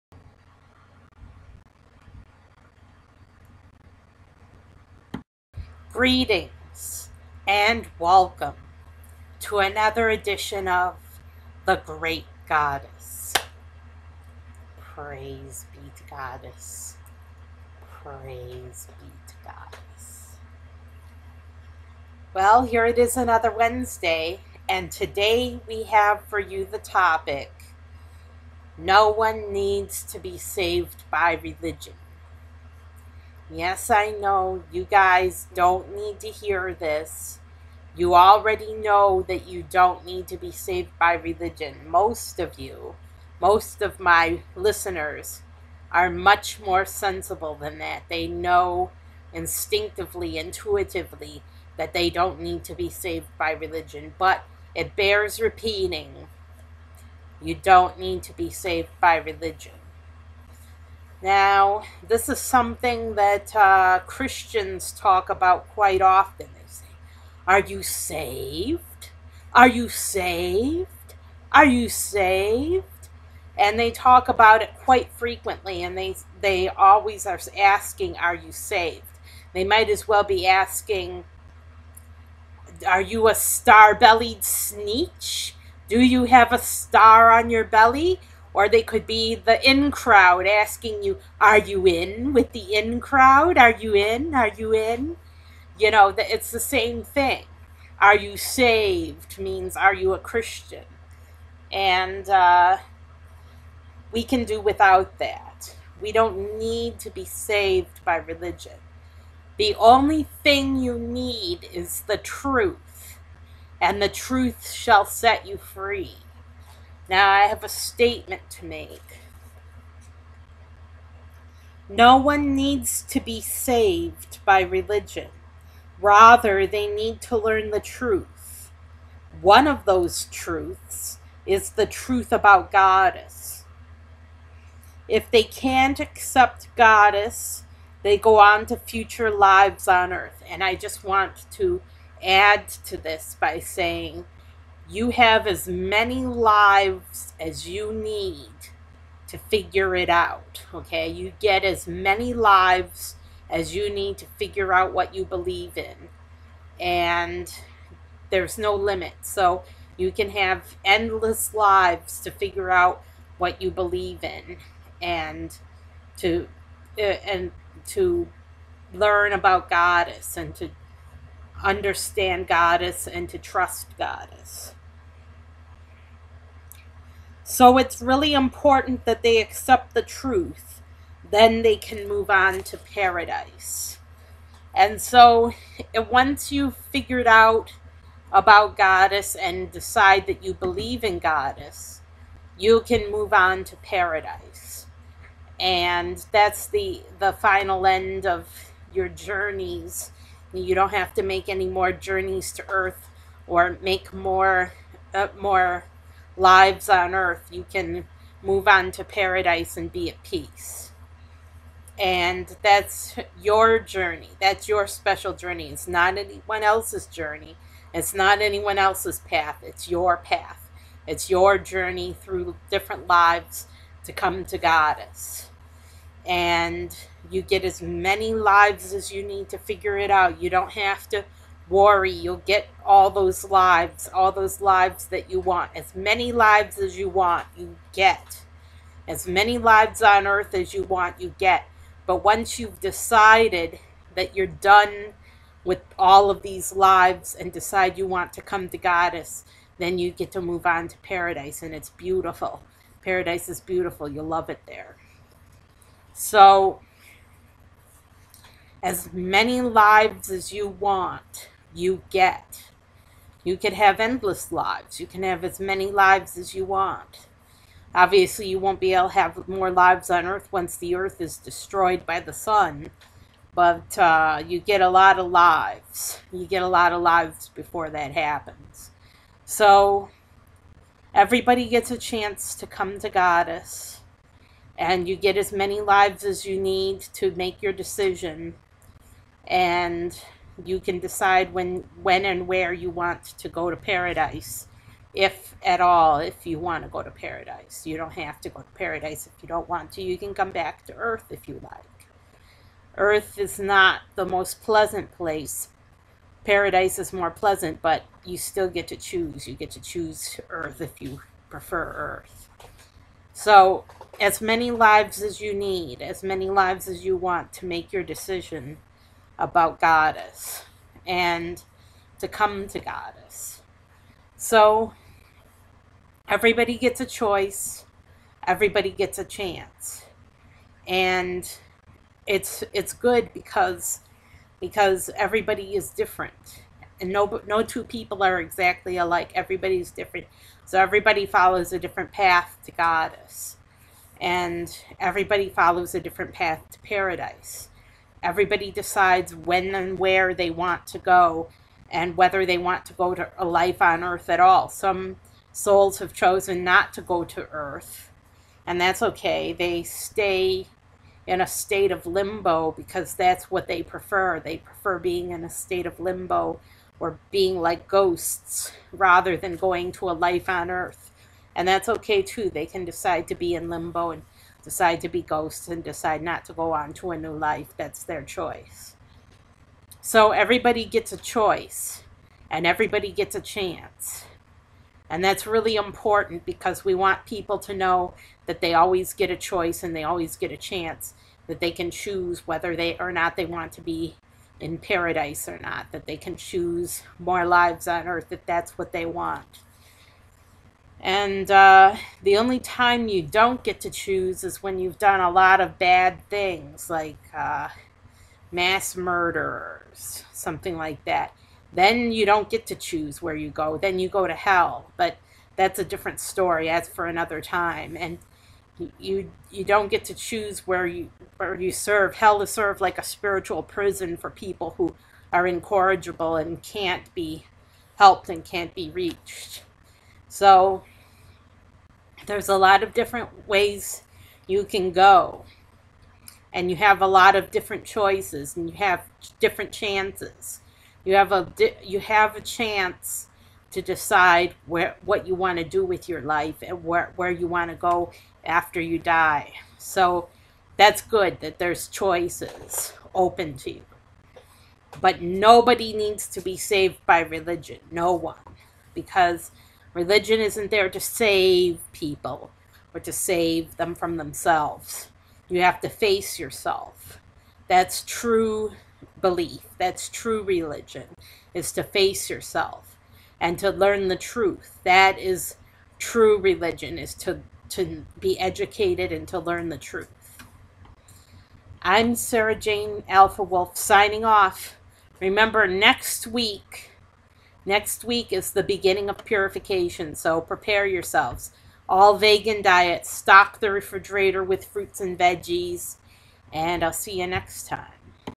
No one needs to be saved by religion - The Great Goddess
No one needs to be "saved" by religion. It should seem obvious, right? No one needs saving. Everyone is on the path that is perfect for him/her. Yet, some people need an explanation. Some people believe everyone needs to be "saved". Examples of this mentality include star-bellied Sneetches and being "in" with the in crowd. In this video, I explain why.